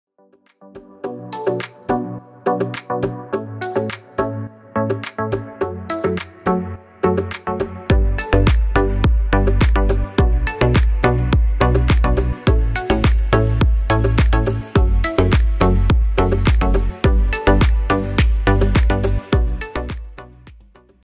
Quick Looks | Sherri Hill | Look 49 | Spring 2023
Embellished Pink Strapless Mini Dress (Look 49) from the Spring 2023 Collection by Sherri Hill.